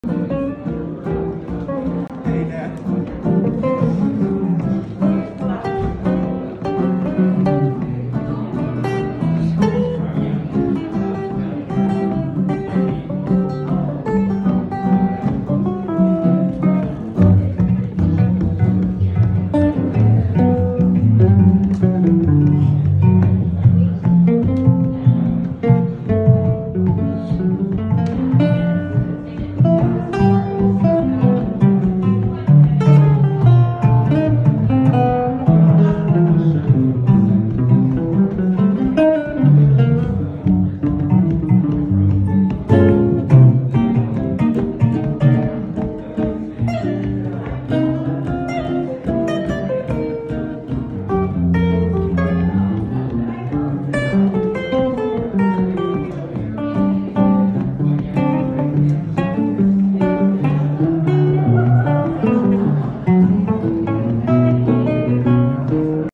Preston Jackson and John Miller
Antique Guitars, featuring Preston Jackson and John Miller, at the Contemporary Art Center of Peoria. January 22, 2022